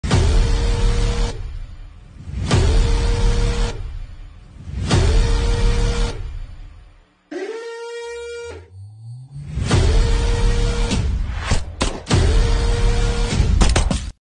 Up close and personal with the world’s most advanced fighter jet 🛩️🌎
The F35 is a true marvel of modern engineering – and it’s just as stunning up close as it is in action 🤩

Shot by- Sony A7IV
Lens- SONY 200/600mm